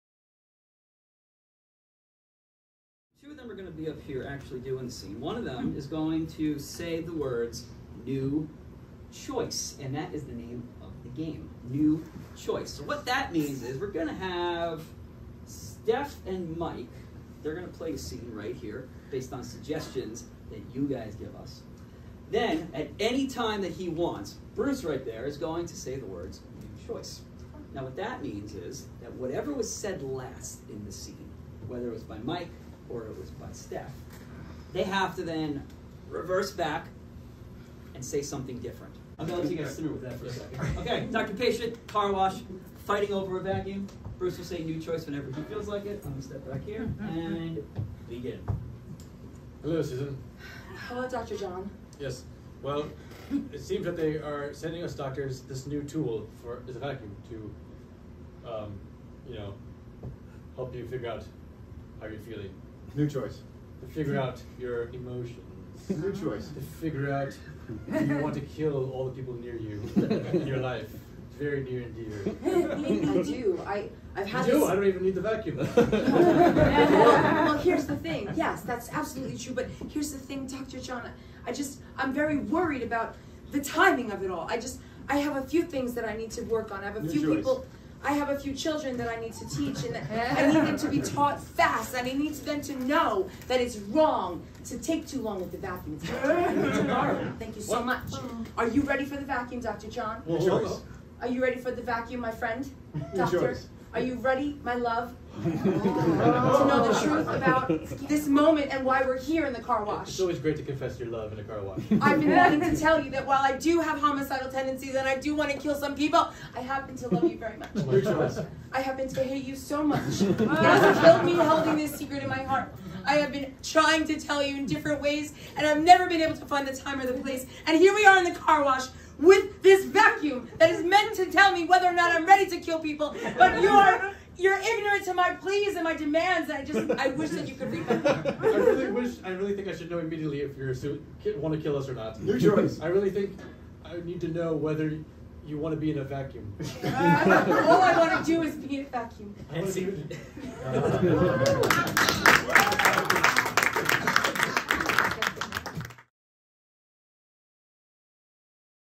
Improv with MPROV! | “New Choice” | promo 5
MPROV, the improv troupe of the Malverne Community Theatre, plays the game “New Choice”.

Featuring:
Michael Pagano
Bruce Liby
Stephannie Jablonsky
Host: Dave Coonan